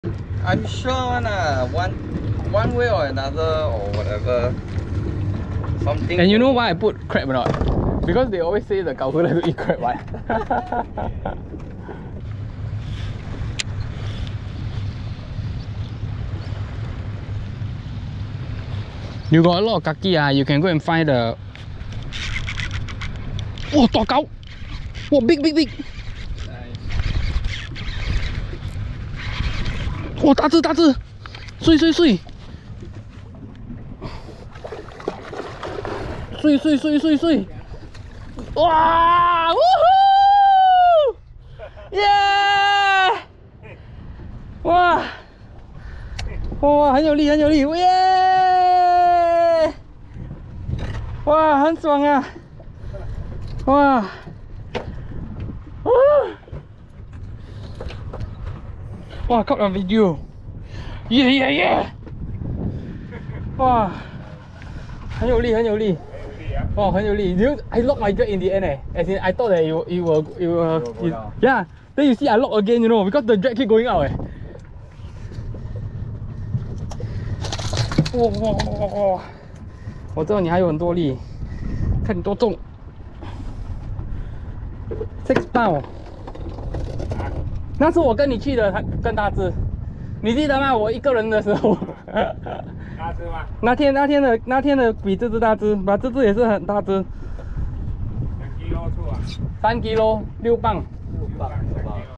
Changi Grouper Luring on BOAT! BIG GROUPER!
Rod - Cast Division Beaststick 69HDG
Reel - 13 Fishing Concept A3 Gen 2 - MODDED with CZB bearing
Line - Gaudis Excelsior X8 pe 4
Lure - Norman
Snap - Fishing Fighter Tetsuwan Snap

- Online fishing tackleshop - Shipping worldwide